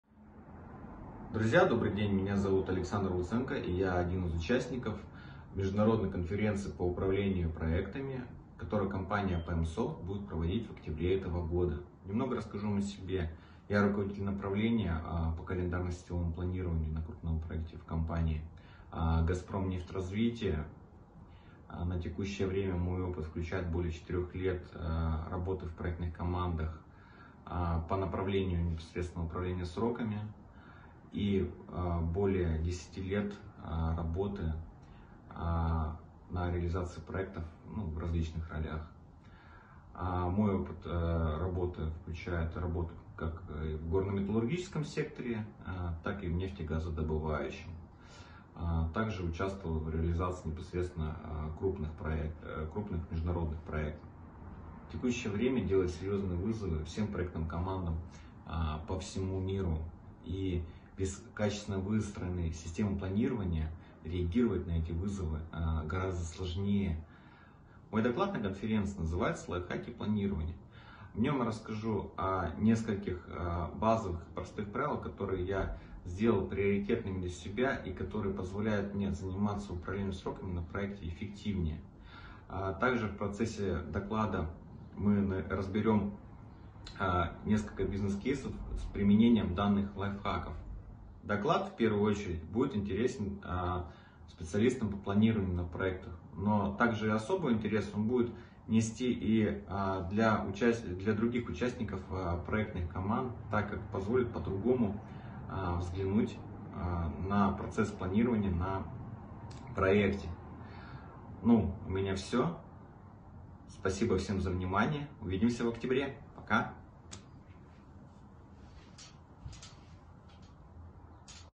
Александр Луценко, ООО "Газпромнефть-Развитие" (Россия)
ООО "Газпромнефть-Развитие", Руководитель направления по календарно-сетевому планированию (направление управление сроками)

Компания «Газпромнефть-Развитие» создана в 2011 году для управления реализацией крупных российских и международных добычных проектов «Газпром нефти».

Коротко о докладе. «Лайфхаки планирования»
Текущее время делает вызовы всем проектным командам по всему миру.
Качественно выстроенные системы планирования реагируют на эти вызовы проще и быстрее.
Есть правила/принципы с помощью которых управлять сроками можно эффективнее. Разберем бизнес кейсы, где вы увидите реальные примеры применения лайфхаков.